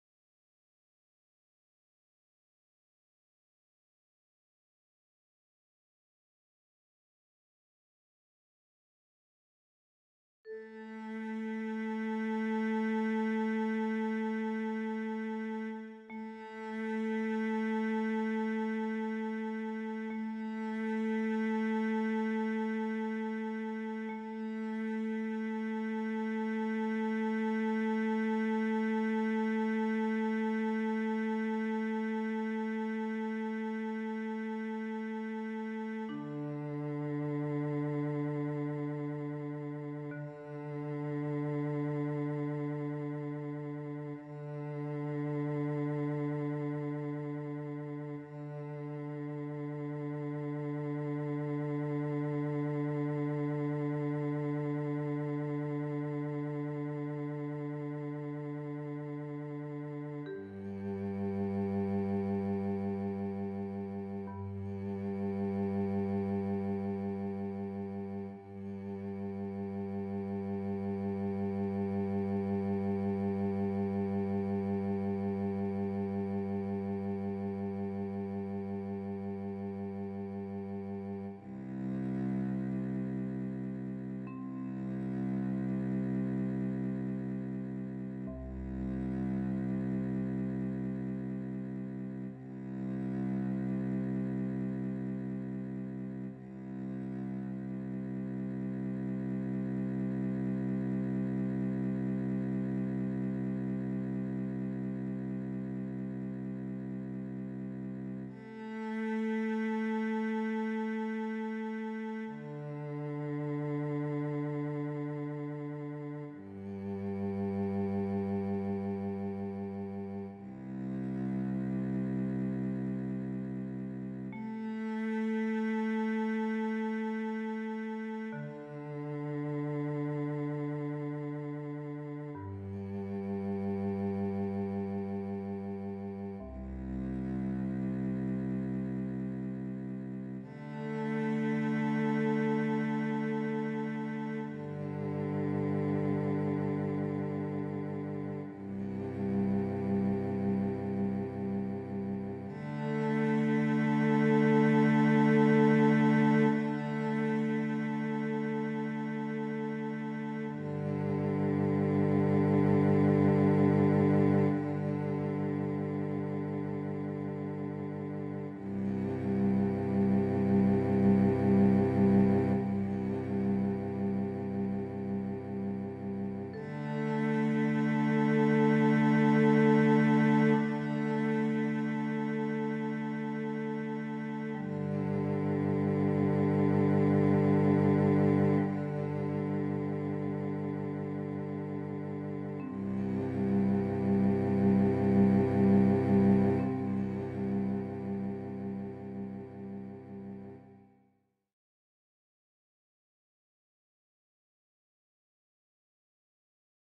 Tuning up a cello
Simply notes to tune a Cello:

A, D, G and C